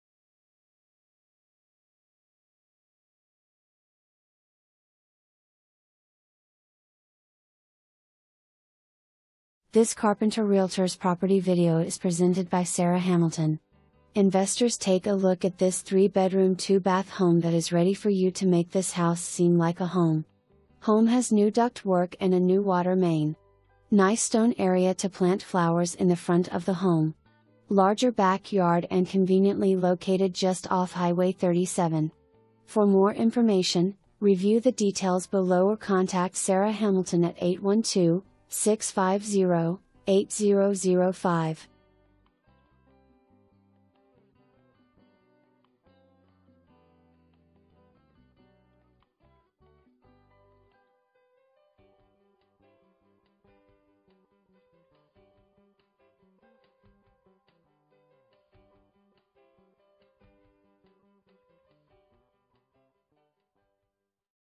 Homes for Sale - 1050 S 9th Street, Mitchell, IN
1050 S 9th Street
Mitchell, IN 47446

Investors take a look at this 3 bedroom 2 bath home that is ready for you to make this house seem like a home. Home has new duct work and a new water main. Nice stone area to plant flowers in the front of the home. Larger back yard and conveniently located just off Hwy 37.

Presented By:
Sarah Hamilton, Carpenter Realtors